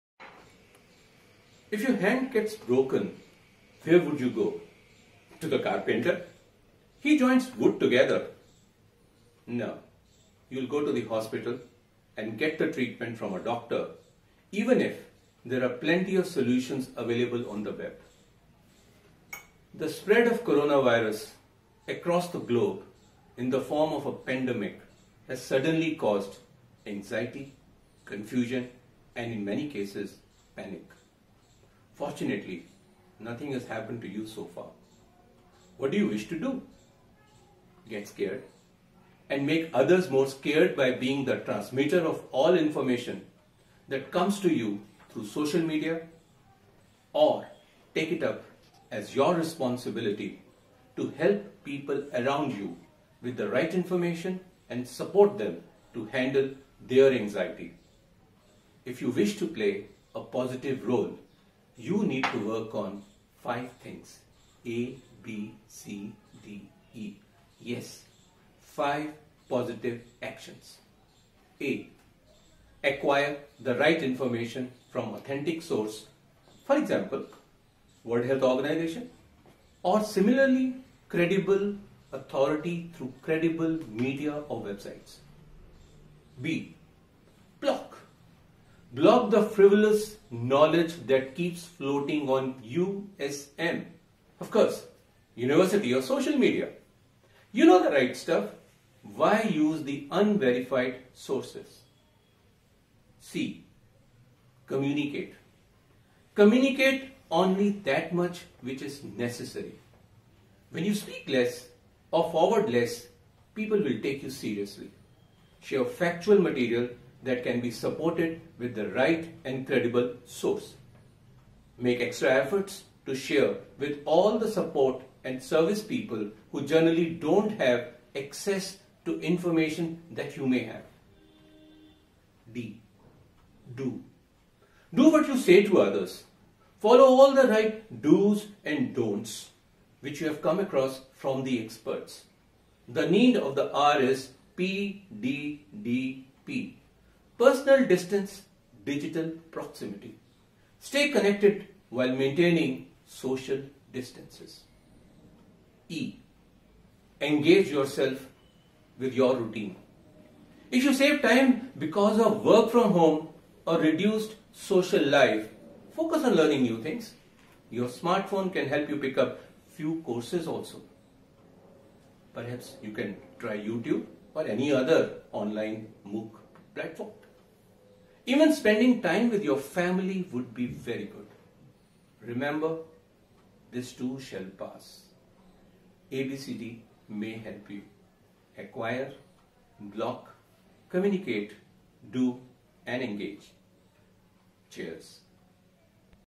Corona ABCDE Actions
Positive Actions we all must take to handle anxieties and contribute meaningfully during this tough challenging time.